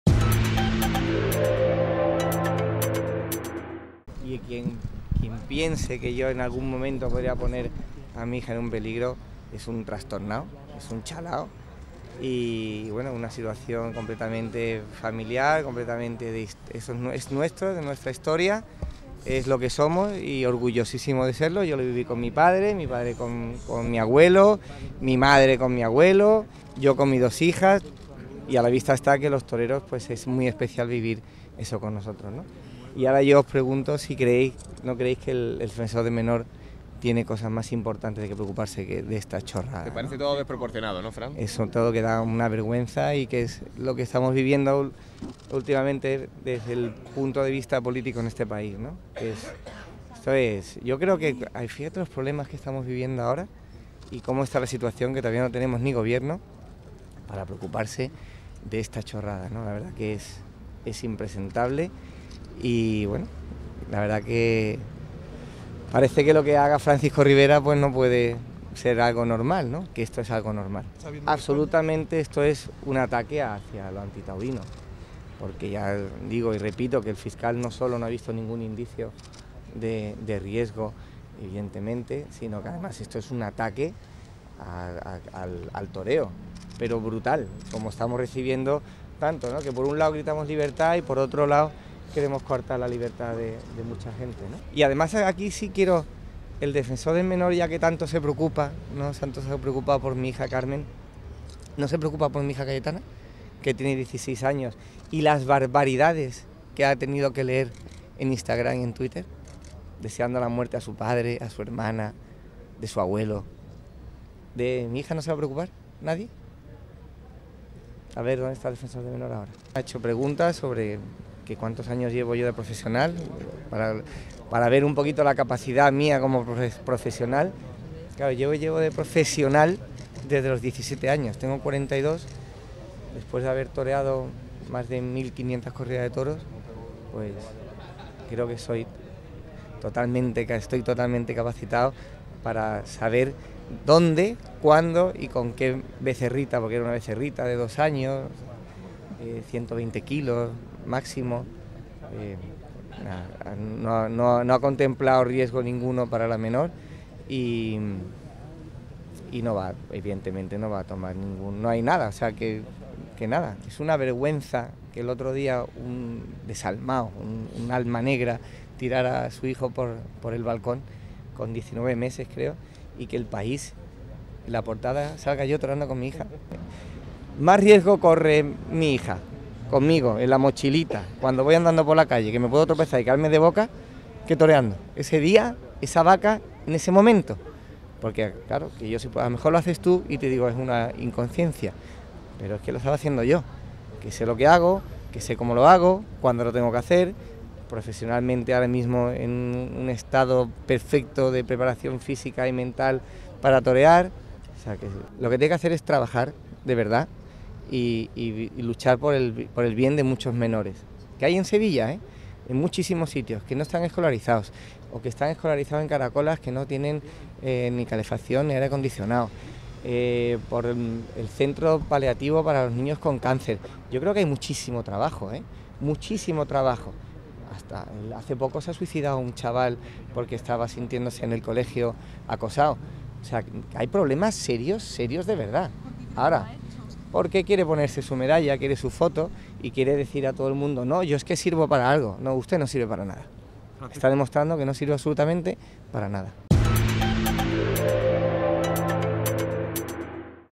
Fran Rivera se defiende y ataca al Defensor del Menor: "Usted no sirve para nada"
El torero Fran Rivera ha comparecido en los juzgados para explicar la foto en la que aparecía toreando con su hija y ha asegurado que es todo una vergüenza y que el Defensor del Menor "no sirve para nada".